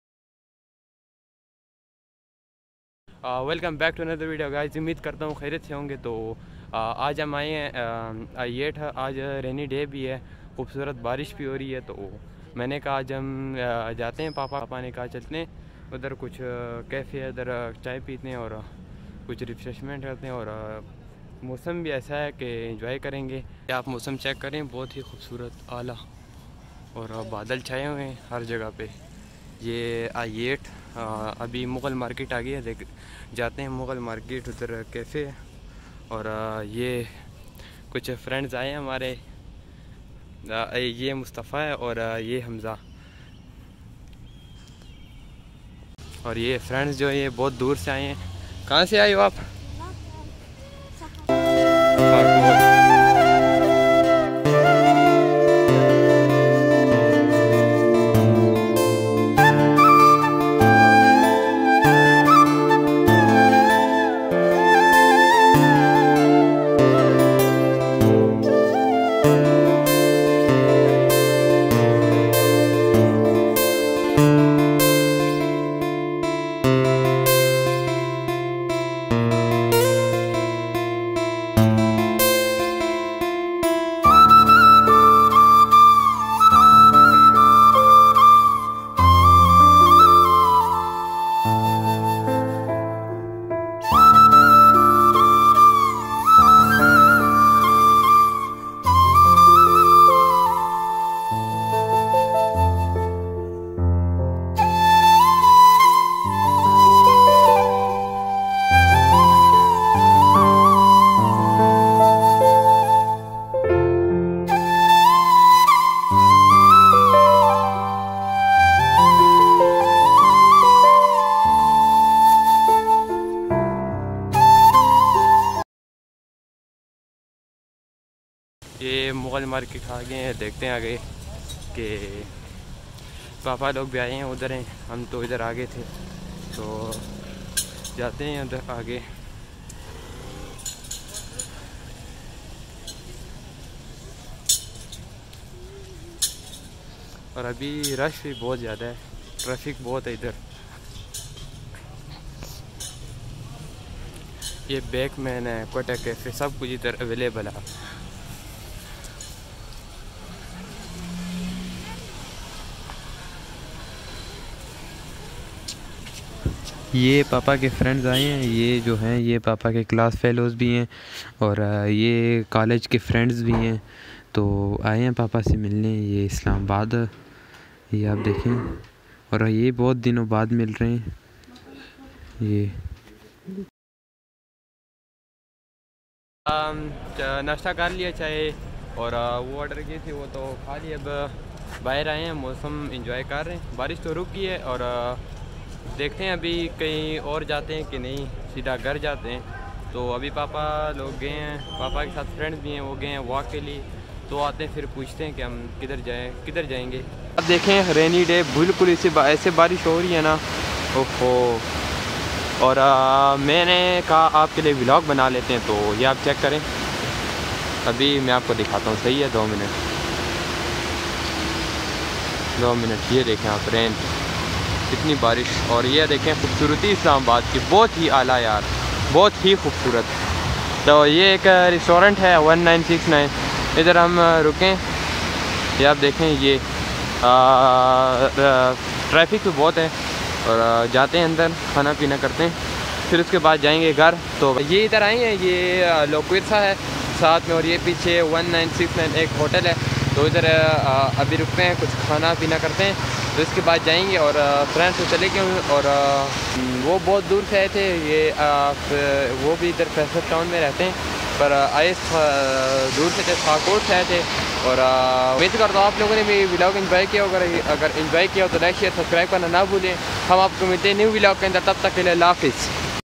EXPLORE ISLAMABAD RAINY DAY MEET WITH FRIENDS (2024) Beautiful weather 😘 enjoy raining
"Islamabad is one of the most beautiful capitals of the world."
Islamabad – the capital city of Pakistan, is one of the most beautiful cities in the South Asian region. Wide, tree-lined streets adorn the various sectors and zones of the city, making it accessible and spectacular.Beautiful weather 😘 enjoy raining
EXPLORE ISLAMABAD RAINY DAY MEET WITH FRIENDS (2024)